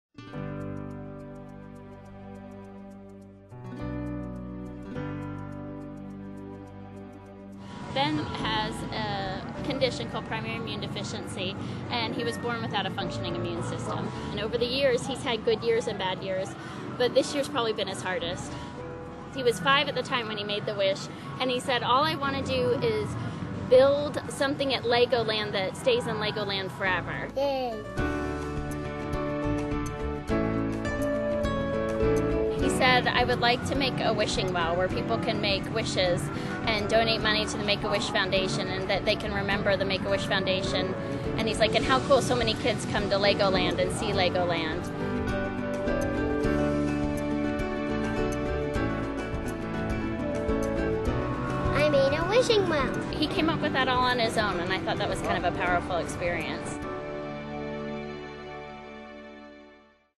Ben's LegoLand Wish
Ben, 6yrs old
Wished to build a wishing well that stayed in LEGOLAND, Florida forever!